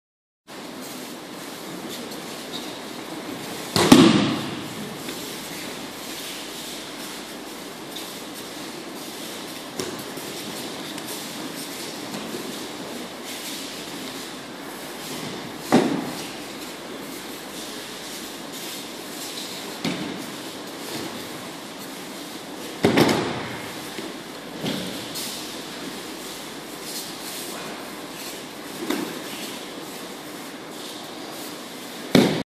Aikido Kaeshi waza
Dror and Vadim
Yokomen Uchi Kotegaedhi Kaeshi Waza
Masatake Dojo Israel